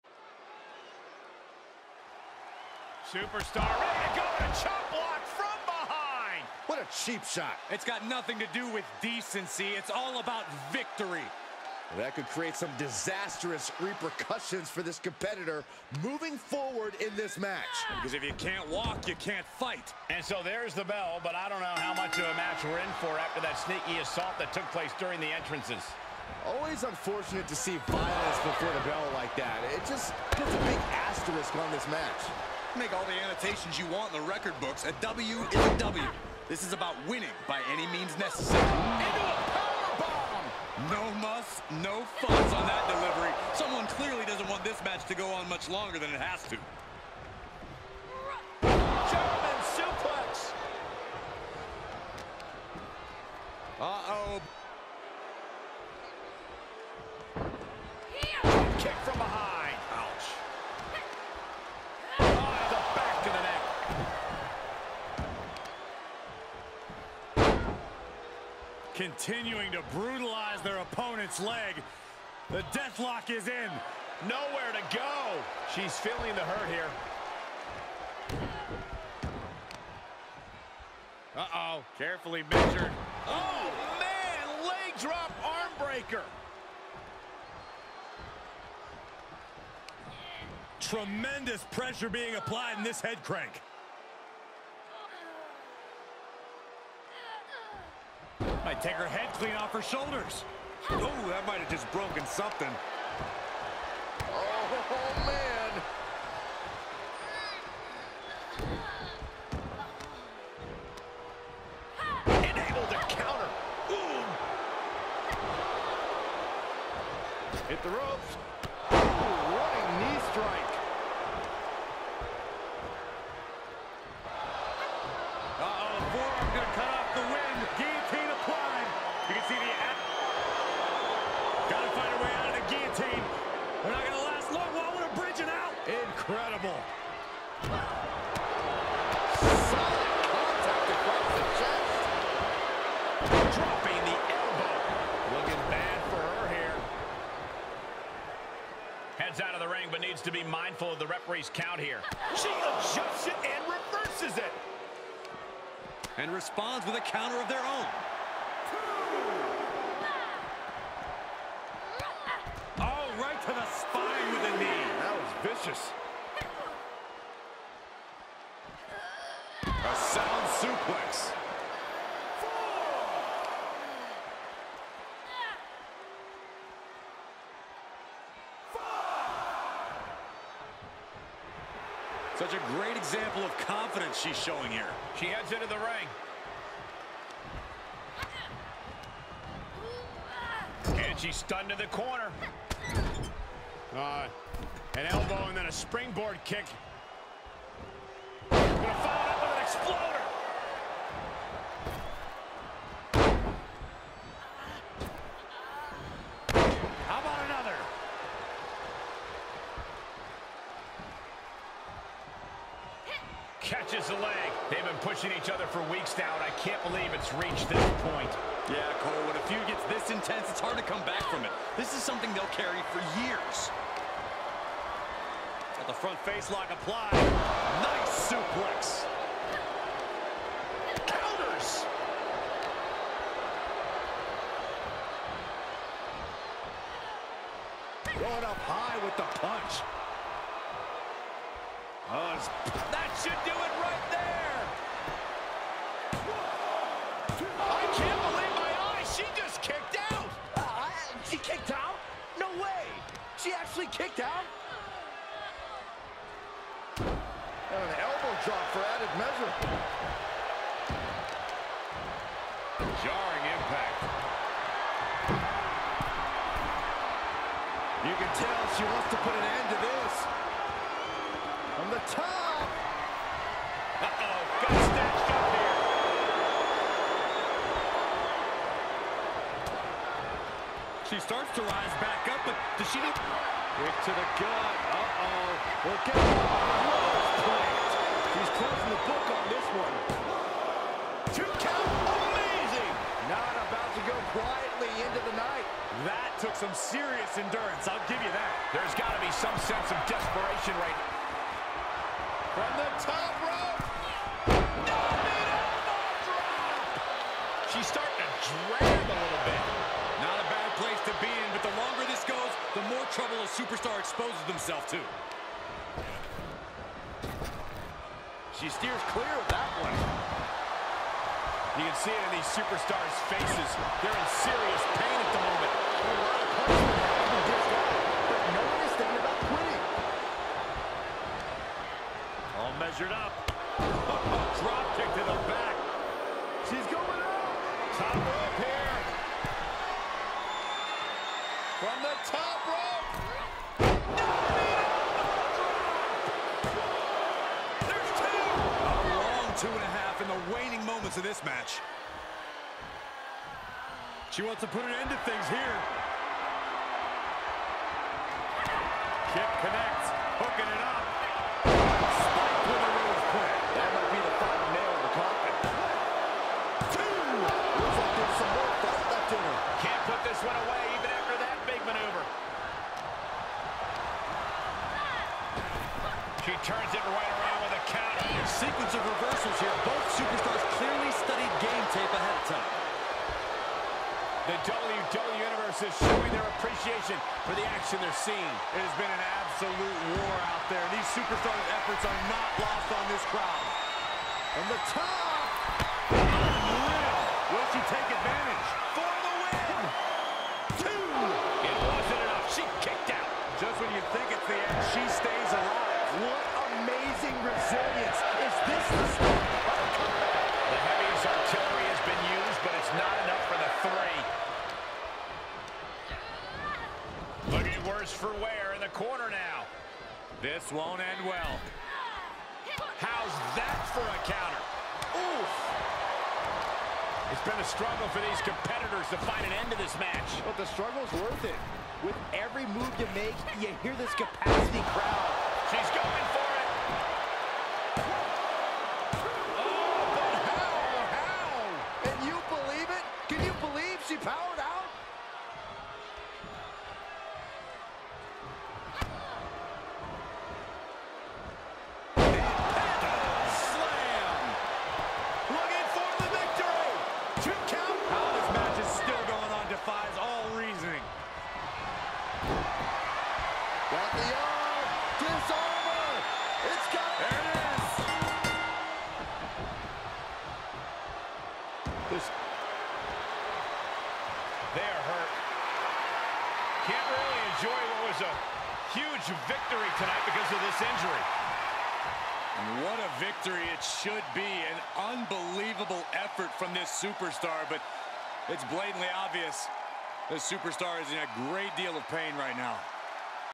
Brawl 46: SEMIFINAL CONTENDER'S MATCH Valencia vs Alice Davenport
Alice Davenport finds a camera backstage and says, "You're gonna need a lot more than a chair shot to keep me down, Val. You know the truth and you're scared. You know if you face me at 100% in this Semifinal Contender's Match, you will lose. I will continue to win and earn my way to that title, but you will just have to cling that much harder to that Tag Team Championship. And you will have to sit with yourself knowing that's the only title you're going to hold because you can't get the job done by yourself!"